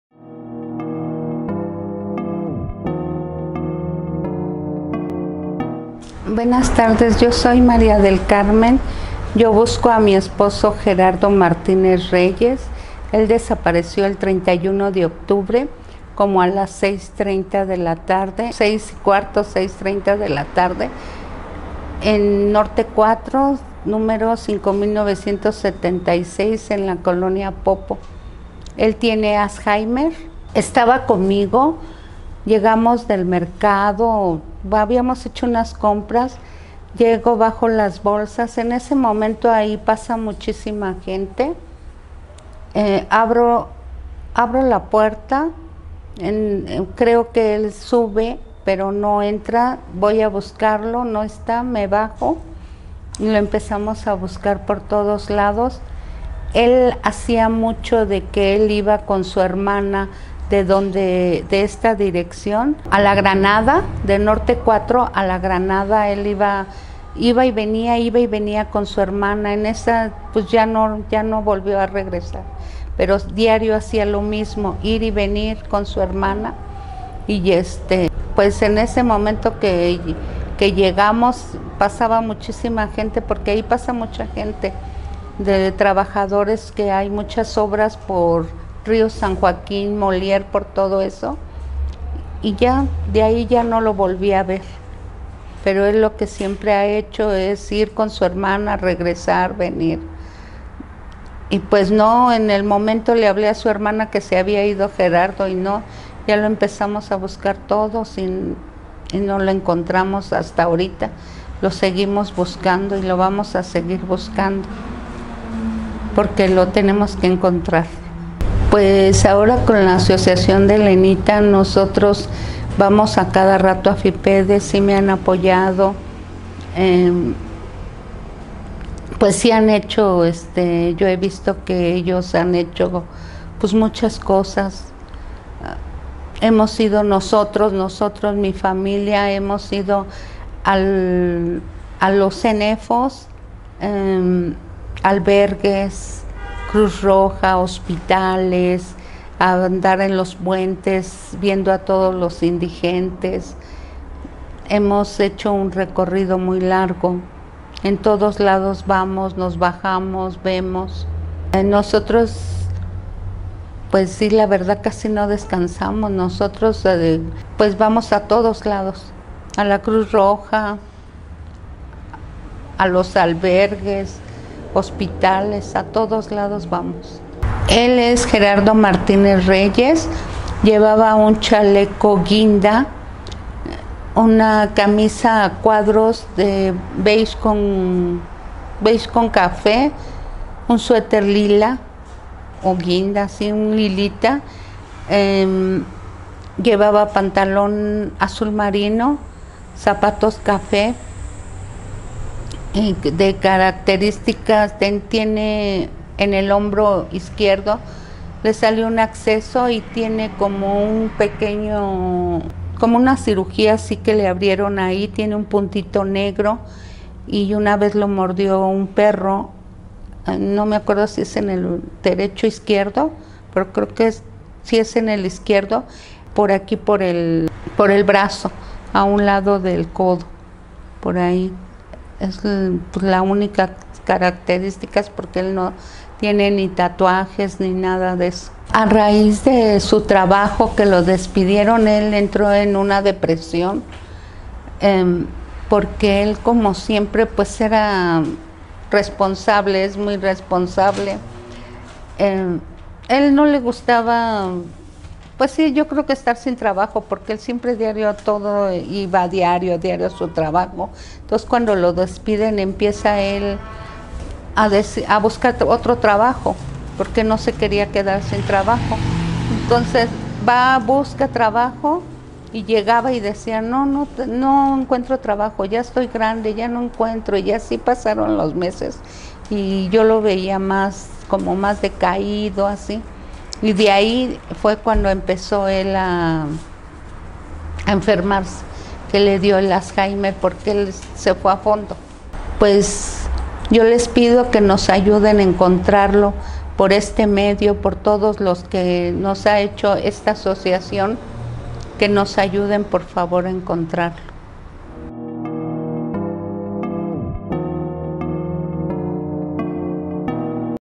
María busca a su esposo Gerardo Martínez Reyes con alzheimer | SE BUSCA
Se pide a la ciudadanía que si saben algo de su paradero, contactarse por nuestro medio o bien comunicarse a través de la Asociación Mexicana de Niños Robados y Desaparecidos A.C. al teléfono 56741902 o bien al correo: o en la pagina web

María Del Carmen Martínez Cortés busca desesperadamente a su esposo Gerardo Martínez Reyes de 60 años de edad, quien desapareció el 31 de octubre de 2019 en Norte 4 y Río San Joaquín, Col. Popo en la alcaldía Miguel Hidalgo en la Ciudad de México.